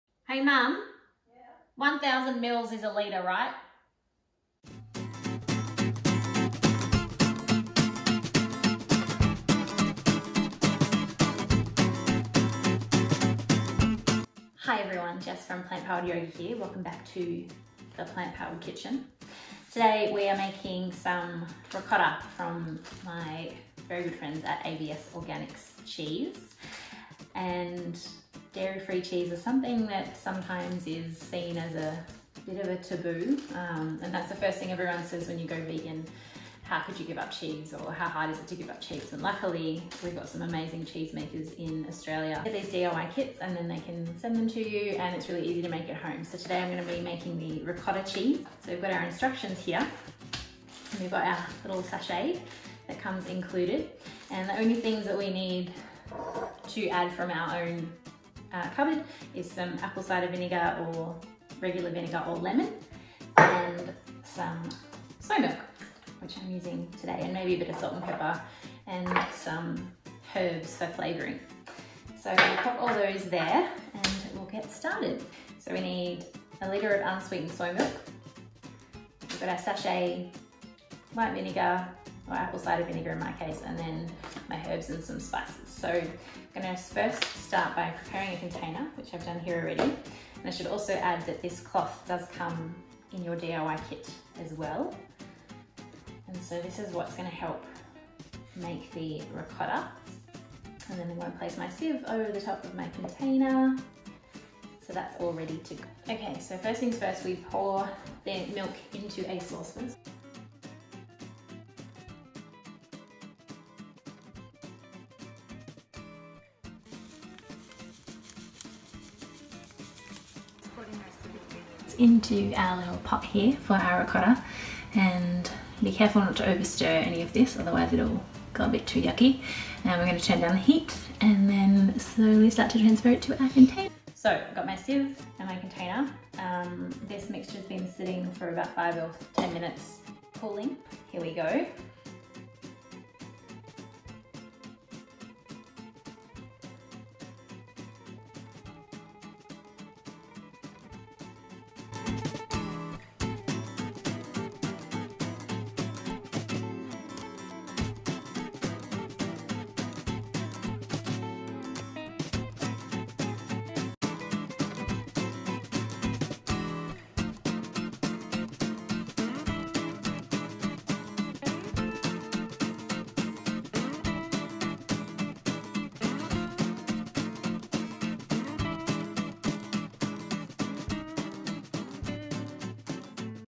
Making Vegan Cheese | AVS Organic Foods
"But how could you give up cheese!?" Ever heard this? As a vegan, it's the most common question (apart from that B12 deficiency) and as the years go on, vegan cheese is becoming more and more accessible.

AVS Organic Foods have their very own DIY Kit that you can make your very own cheese at home with and it comes in handy to have your own supply to add to your own creations at home.

In this video, I make the AVS Ricotta cheese! It was lots of fun and tastes delicious. Have you ever tried making your own cheese?